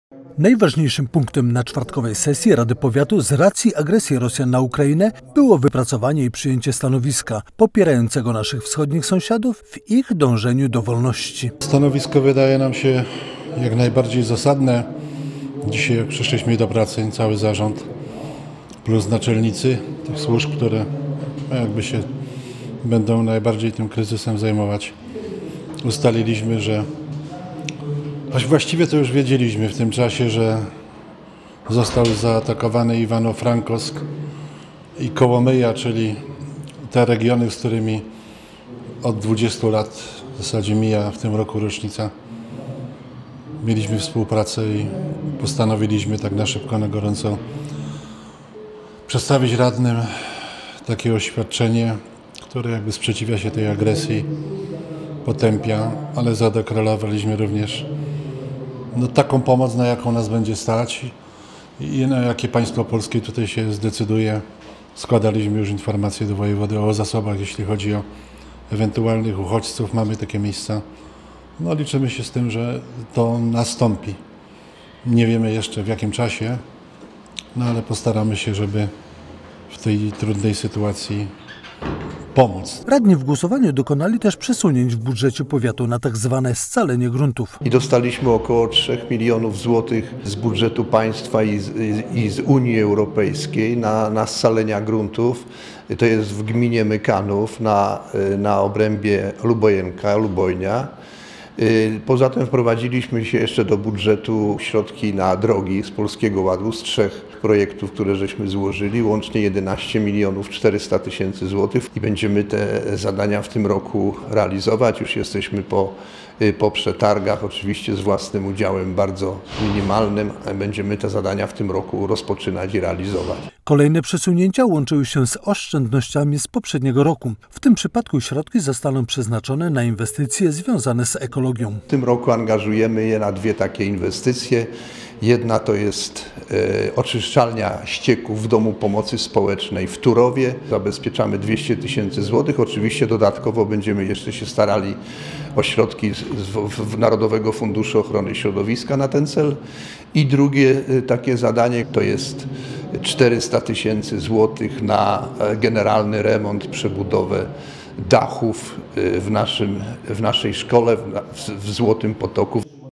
Obradowali radni powiatowi
Zmiany w budżecie i WPF, sprawozdania z działalności poszczególnych komisji Rady oraz informacja o działalności komendanta Miejskiego Państwowej Straży Pożarnej - to niektóre sprawy jakimi zdalnie zajmowali się radni powiatowi.